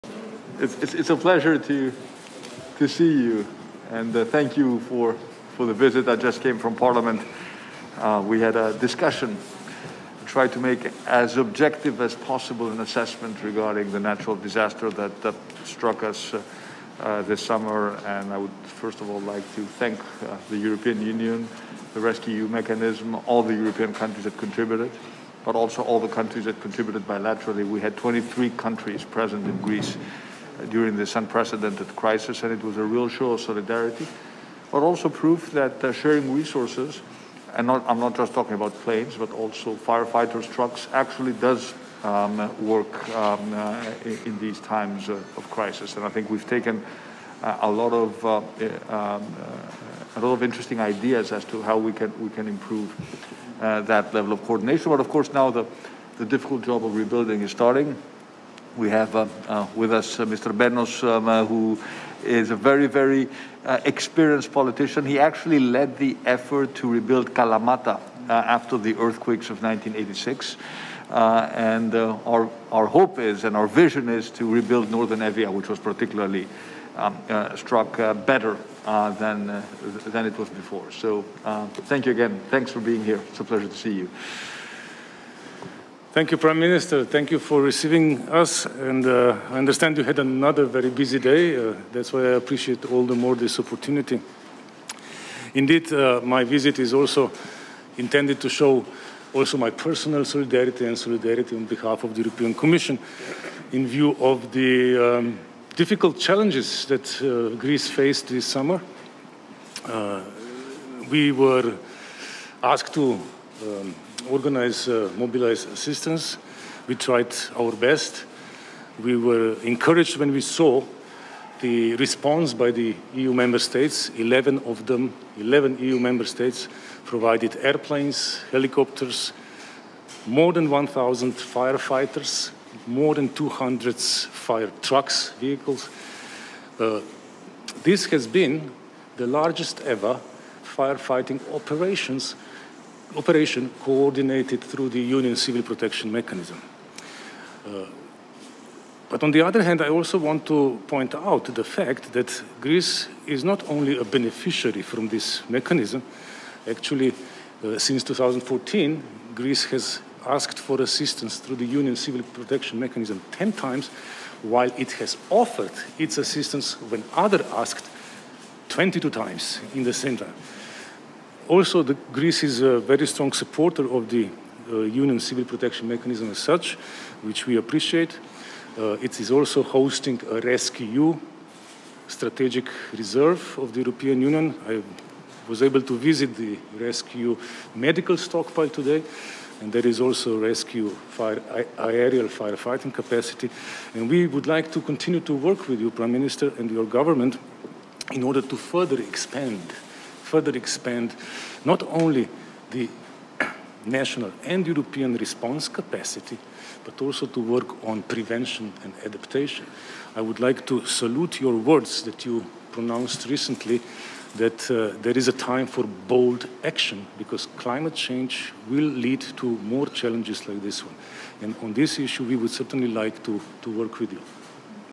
Συνάντηση Κυριάκου Μητσοτάκη με τον Επίτροπο αρμόδιο για τη Διαχείριση Κρίσεων Janez Lenarčič
Ο Πρωθυπουργός Κυριάκος Μητσοτάκης συναντήθηκε το απόγευμα στο Μέγαρο Μαξίμου με τον Επίτροπο της ΕΕ, αρμόδιο για τη Διαχείριση Κρίσεων, Janez Lenarčič.

Στη συνάντηση συμμετείχαν από ελληνικής πλευράς ο Υφυπουργός στον Πρωθυπουργό αρμόδιος για Θέματα Κρατικής Αρωγής και Αποκατάστασης από Φυσικές Καταστροφές Χρήστος Τριαντόπουλος, ο Περιφερειάρχης Κεντρικής Μακεδονίας και Πρόεδρος της ΕΝΠΕ Απόστολος Τζιτζικώστας, ο επικεφαλής της Ειδικής Επιτροπής για την Ανασυγκρότηση της βόρειας Εύβοιας Σταύρος Μπένος, ο Γενικός Γραμματέας Πολιτικής Προστασίας Βασίλειος Παπαγεωργίου και η Αναπληρώτρια Κυβερνητική Εκπρόσωπος Αριστοτελία Πελώνη.